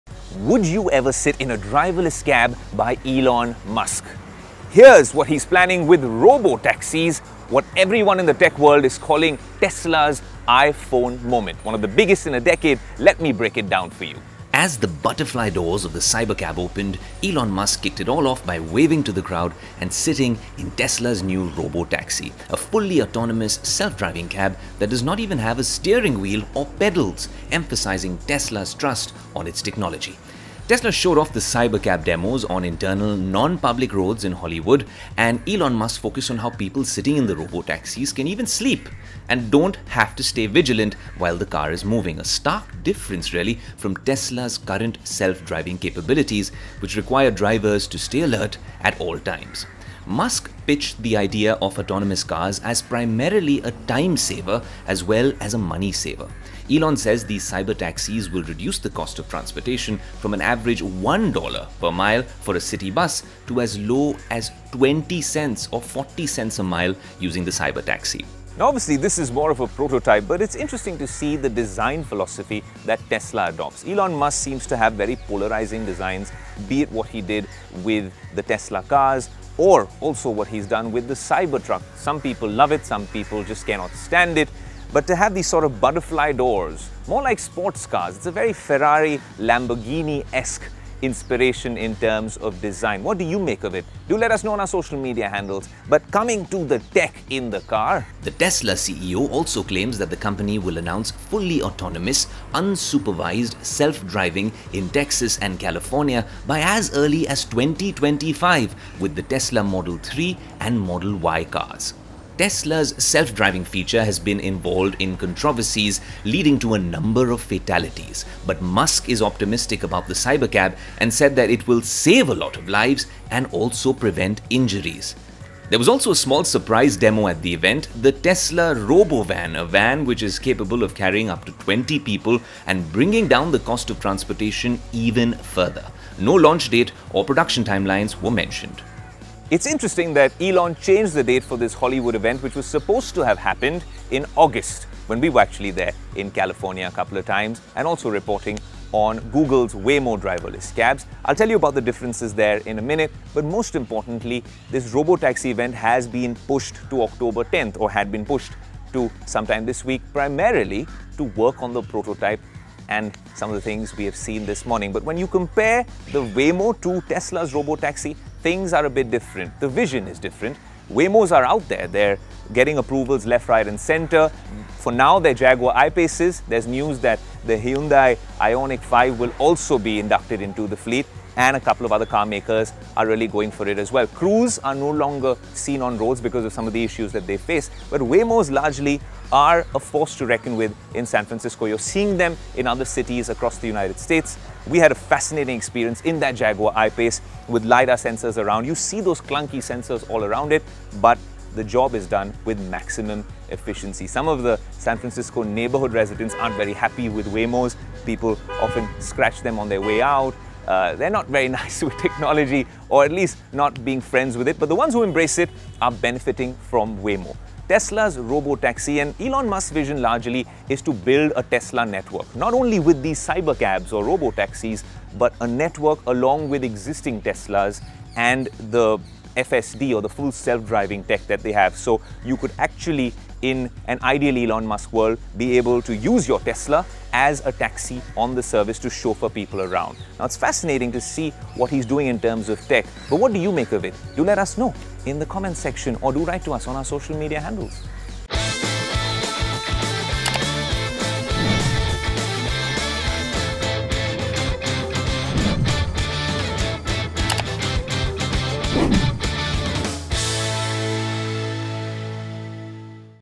Tesla's Driverless Taxi Is Here! All You Need To Know | Tech Today
Elon Musk and Tesla have just unveiled the Tesla Cybercab, a driverless, fully autonomous cab that aims to revolutionise the public transport system. Tech Today’s Aayush Ailawadi tells you all you need to know about the looks and the technology of this new self-driving cab from Tesla.